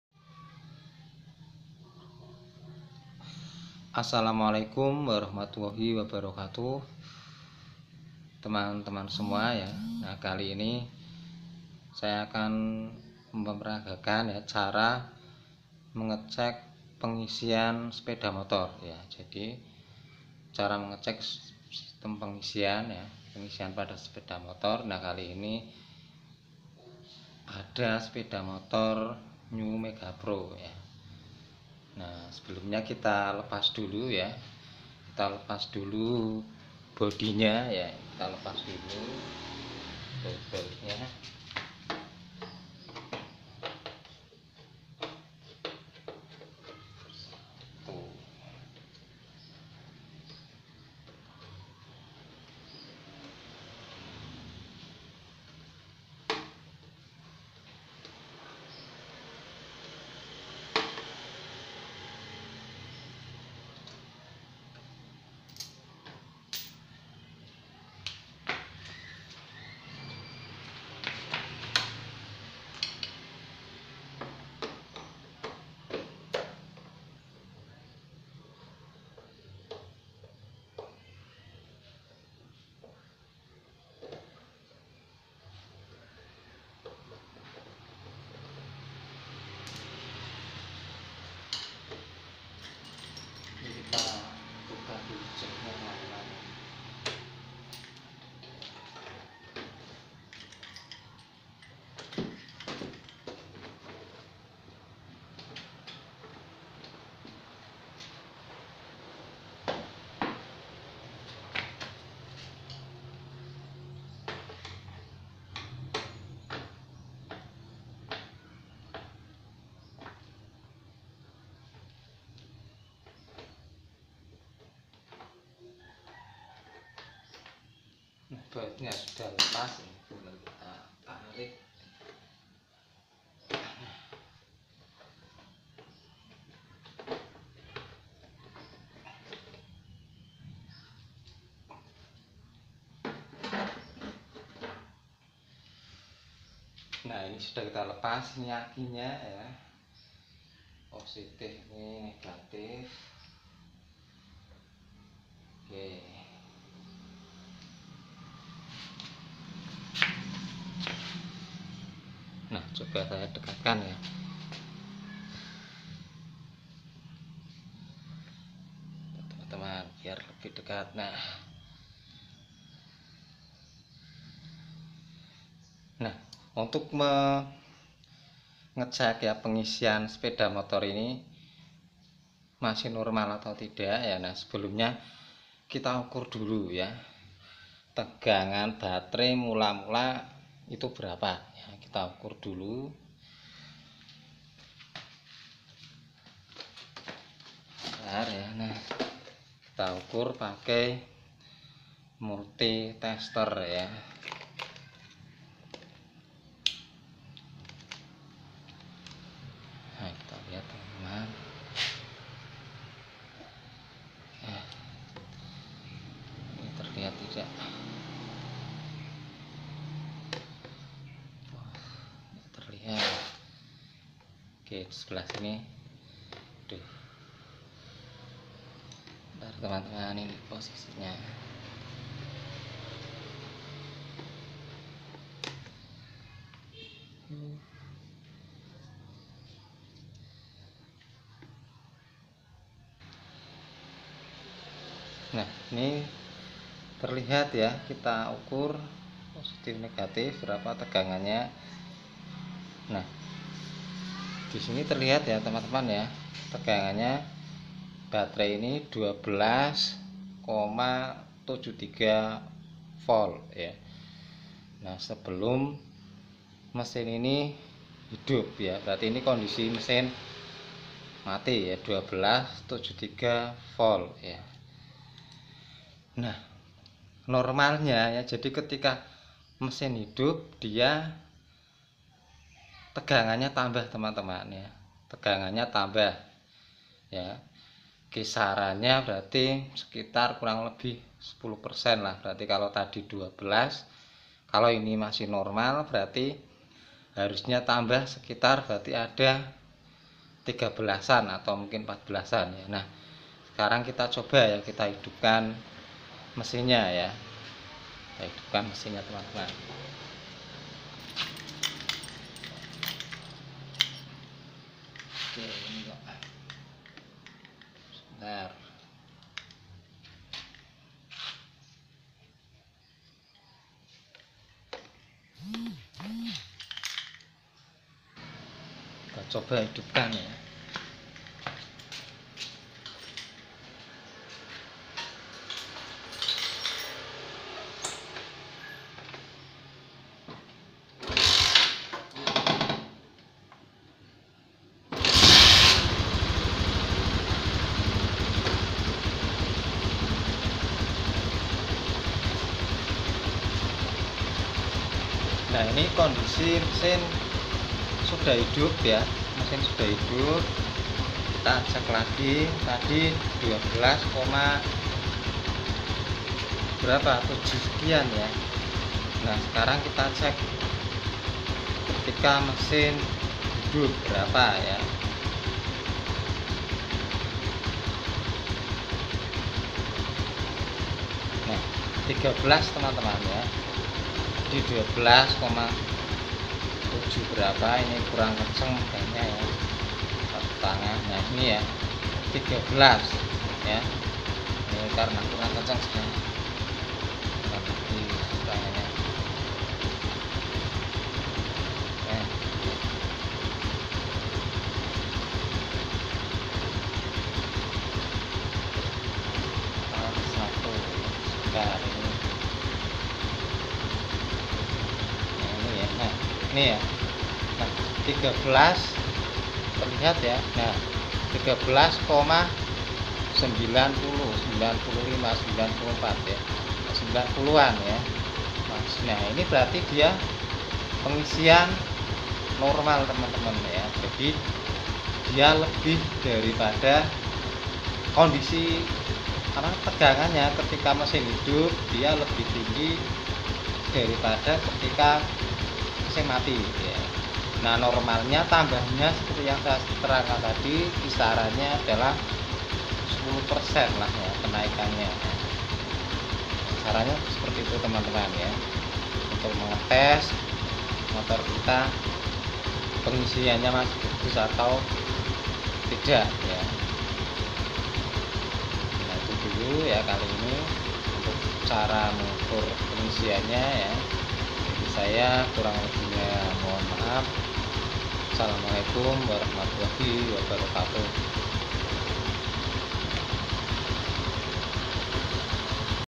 cara memeriksa pengisian sepeda motor || Sinau otomotif online
Video ini menjelaskan cara memeriksa atau mengecek pengisian sepeda motor.

mengenali kunci kontak AC dan DC

generator yang digunakan sistem pengisian sepeda motor menghasilkan tegangan